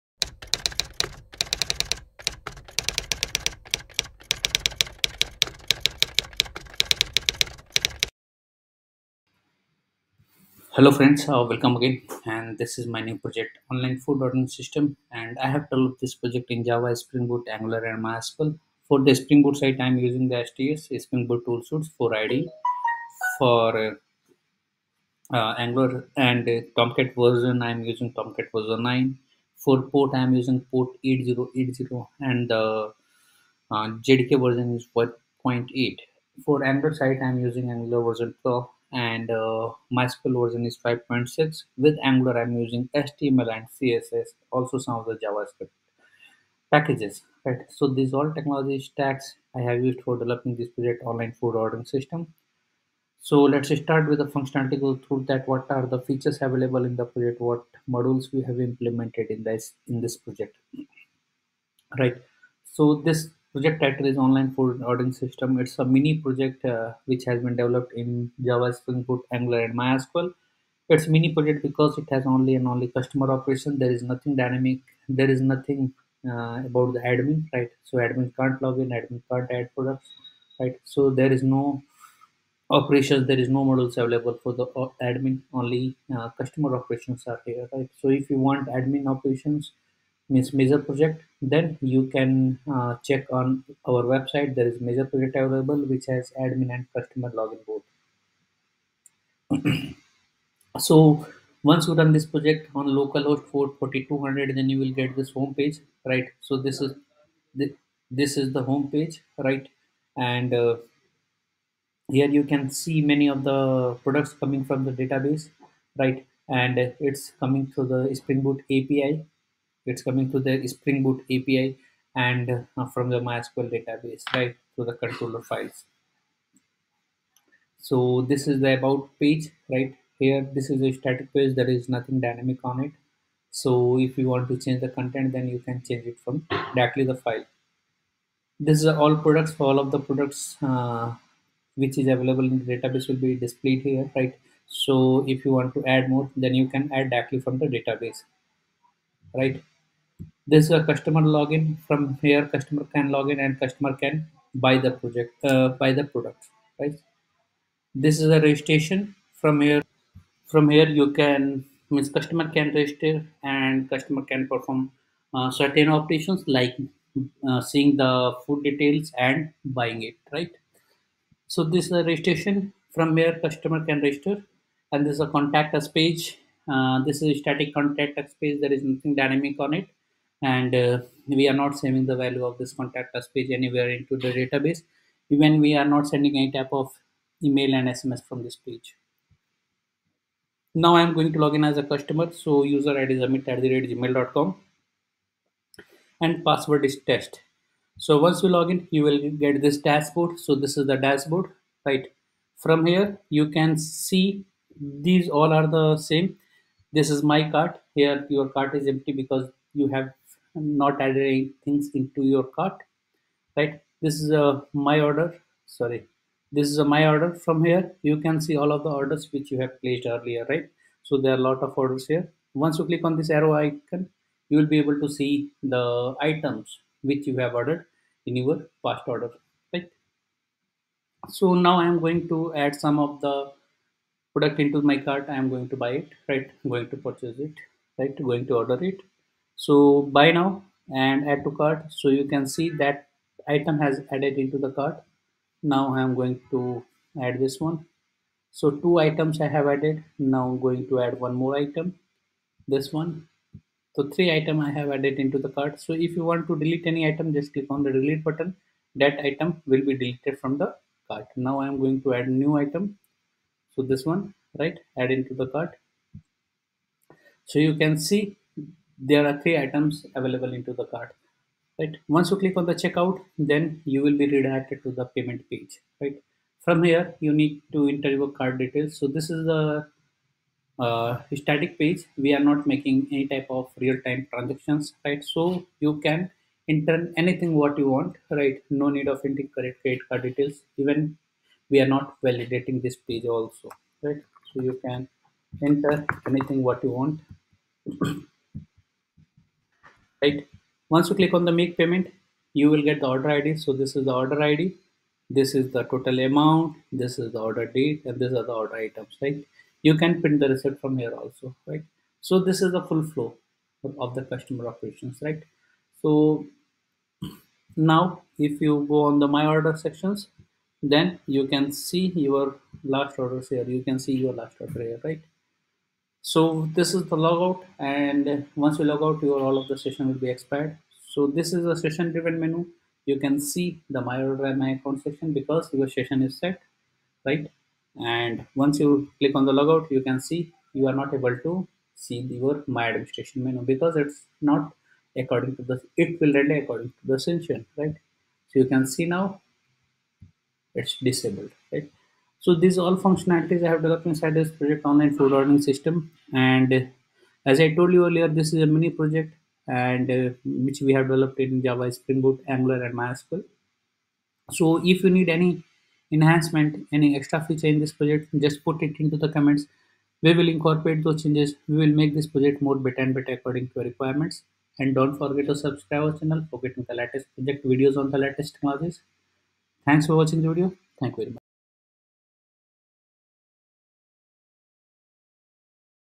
Online Food Ordering System | Spring Boot Angular Project Tutorial | Spring Boot CRUD Project
Online Food Ordering System is a spring boot micro service project where all APIs we have developed in spring boot and all UI Online Food Ordering System  we have implemented in Angular.

Online Food Ordering System Spring Boot Angular Project Tutorial